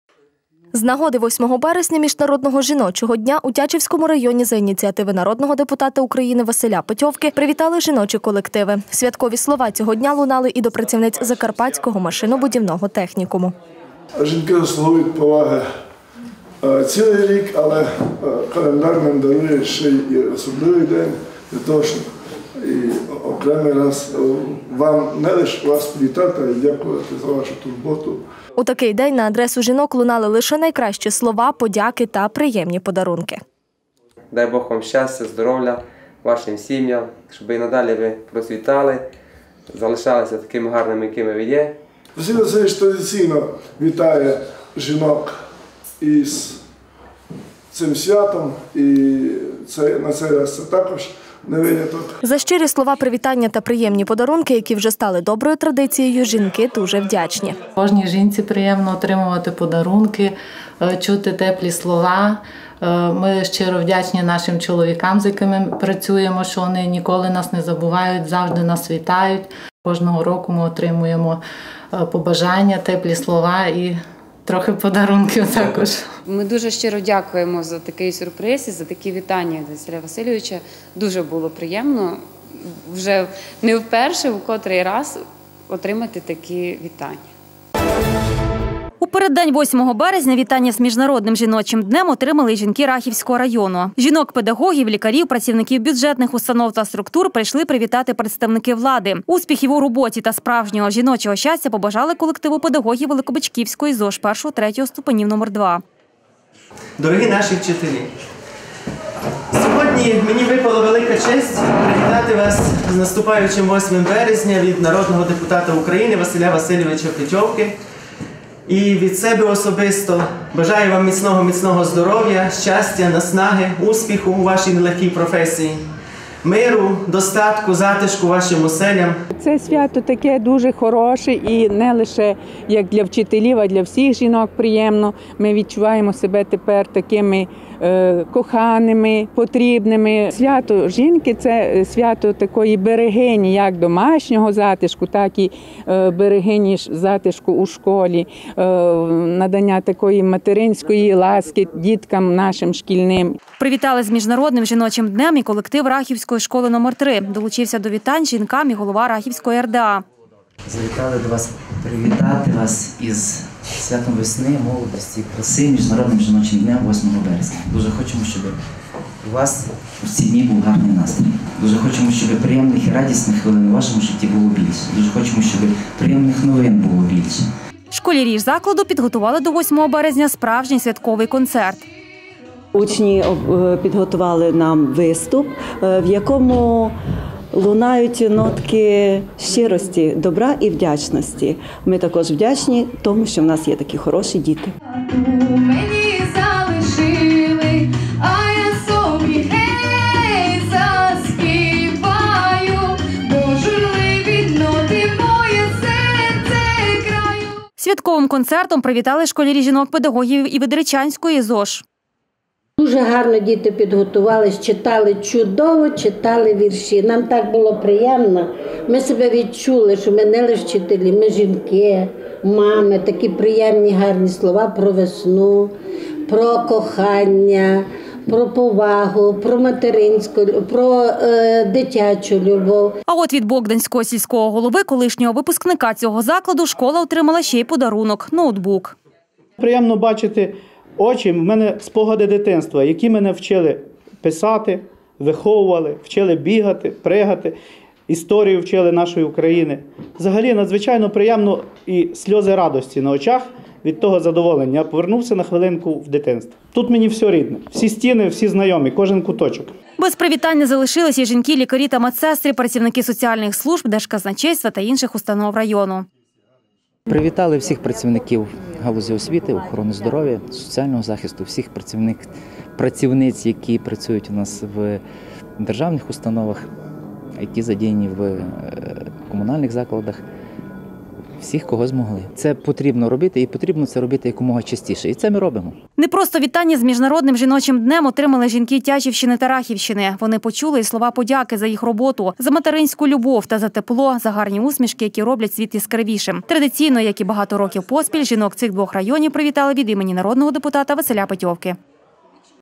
Вітання для жінок Тячівщини та Рахівщини
З нагоди 8 березня, міжнародного жіночого дня, у Тячівському та Рахівському районах  за ініціативи народного депутата України Василя Петьовки привітали жіночі колективи. Не тільки  вітання з міжнародним жіночим днем отримали  жінки Тячівщини та Рахівщини,  вони почули і  слова подяки за їх роботу,  за материнську любов та за тепло та гарні усмішки які роблять світ яскравішим.